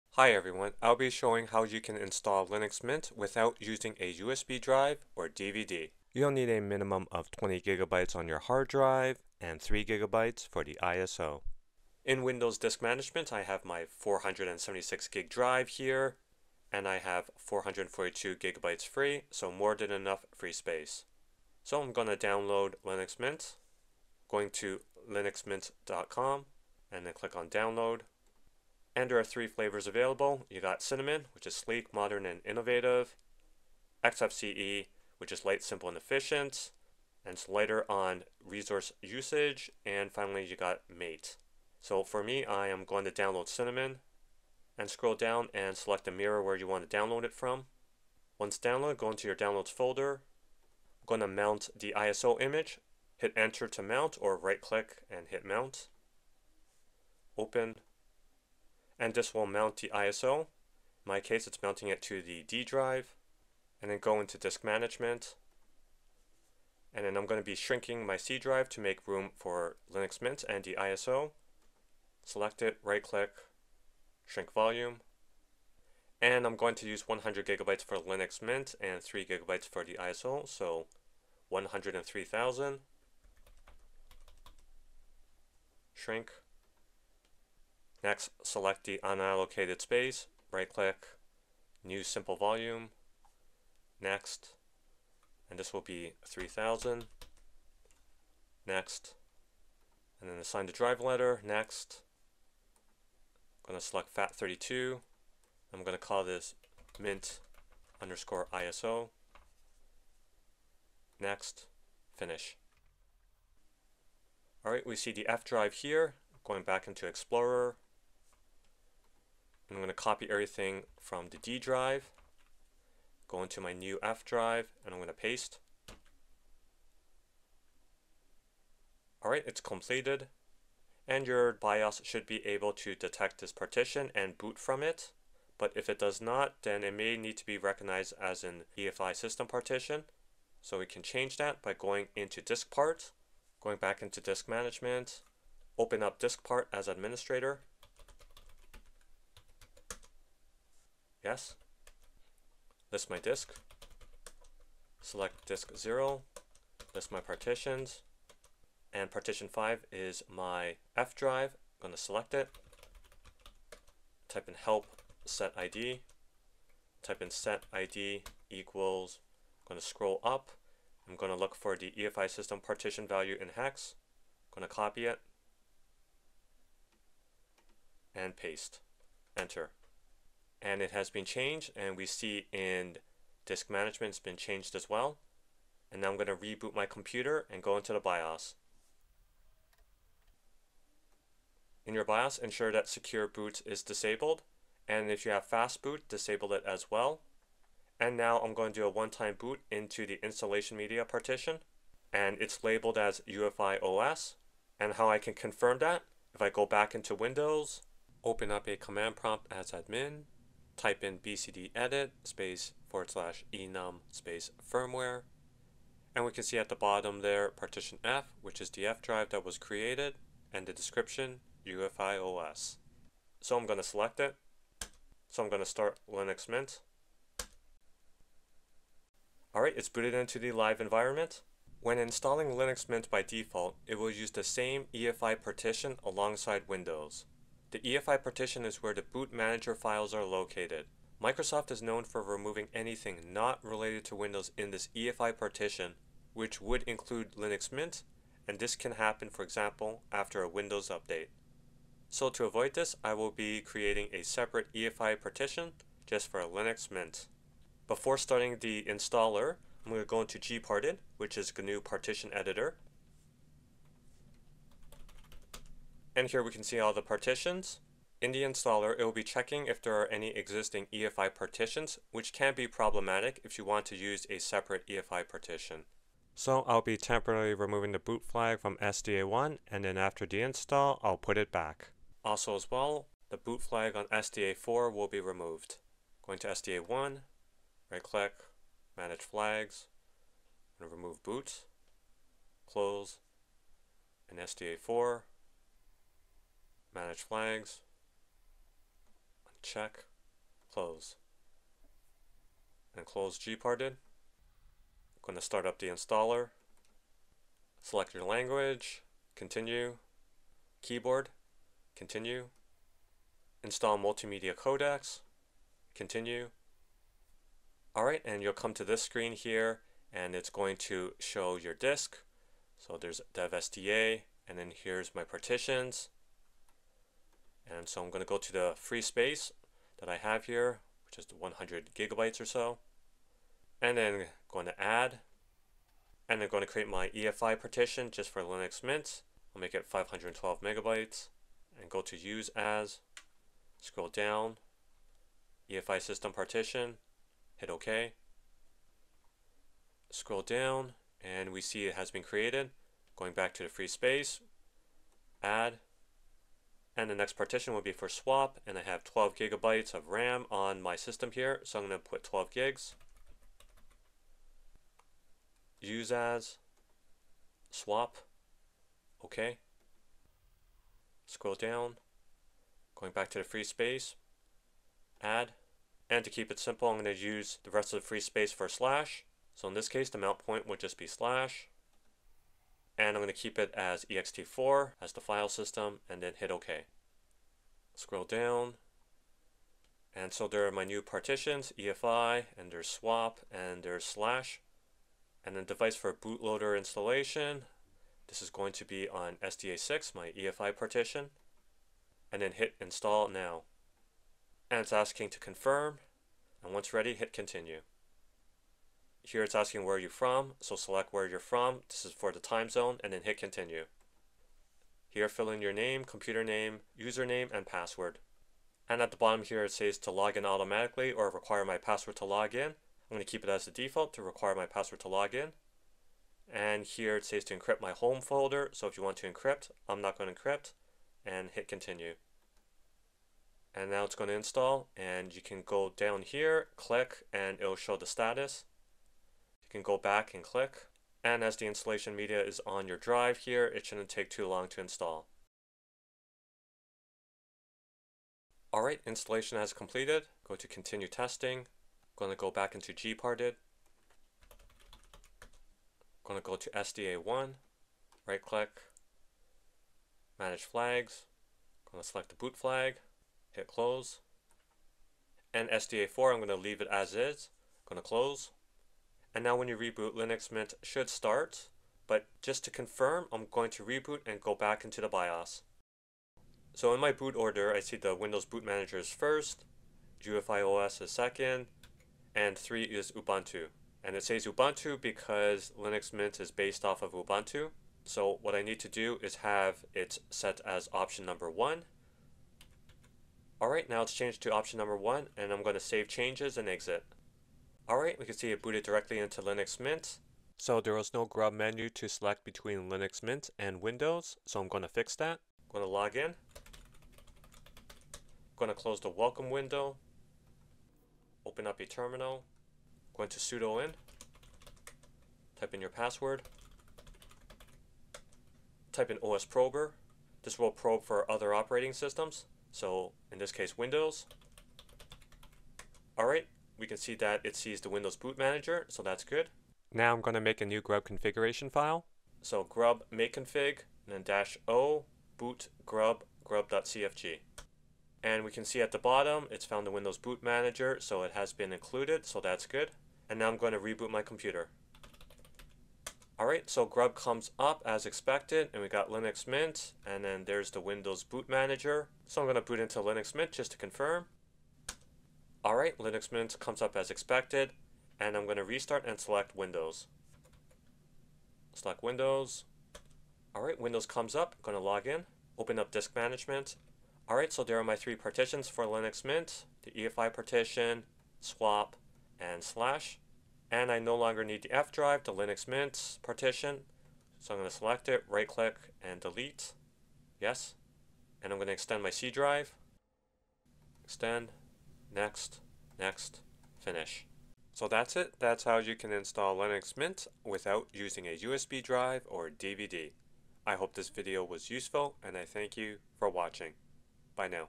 How To Install Linux Mint Without USB Drive | Dual Boot With Windows | UEFI
Learn to install Linux Mint 22.1 without a USB drive or DVD and dual boot with Windows.